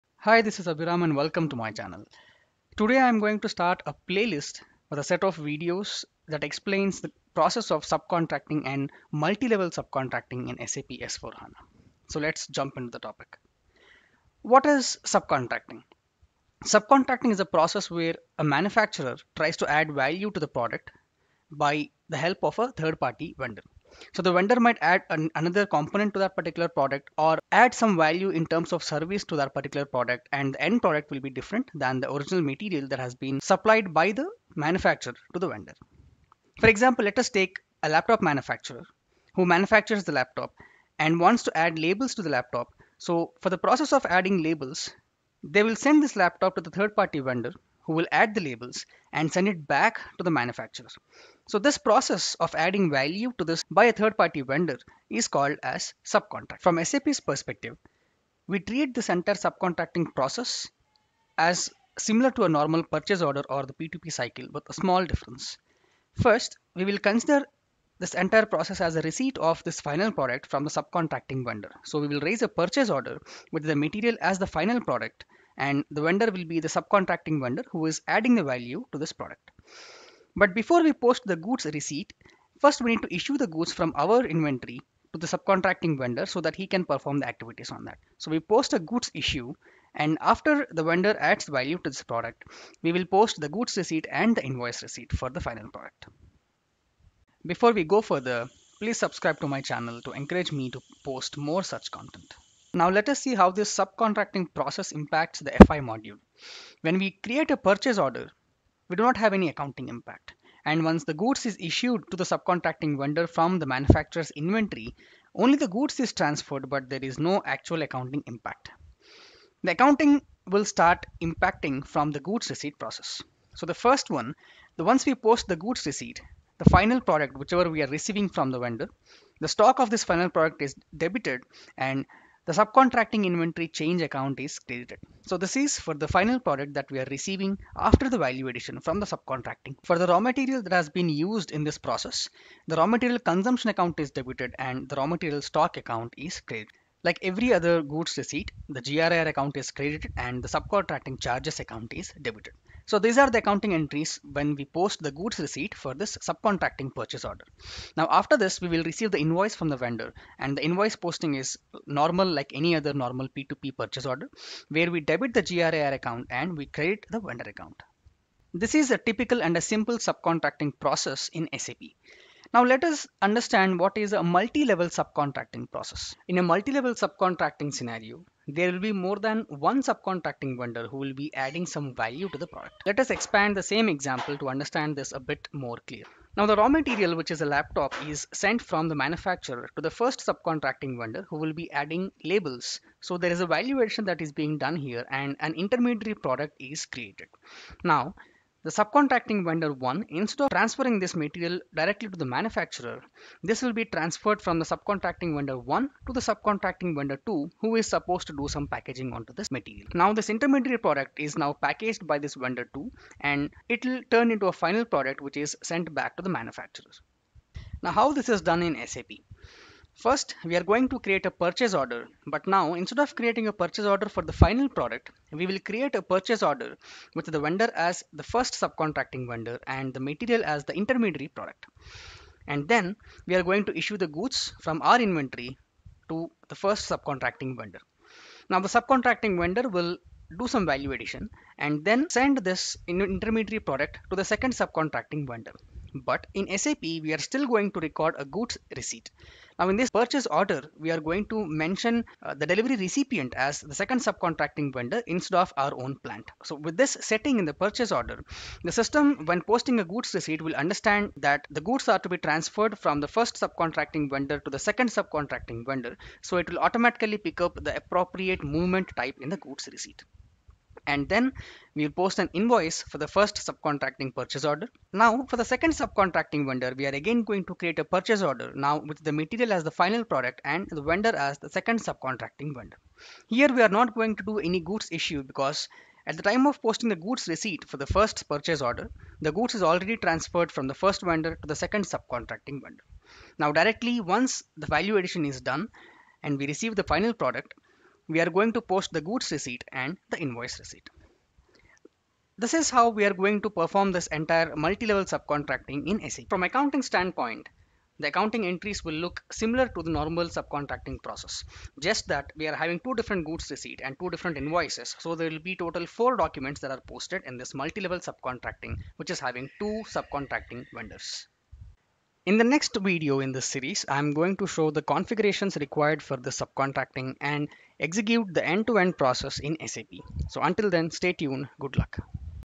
MULTI-LEVEL SUBCONTRACTING | SAP S4HANA | AC SAP CONSULTING
In this video series I am going to explain the concept, configuration and execution of Subcontracting and Multi-level Subcontracting procoesses in SAP S4HANA 2020.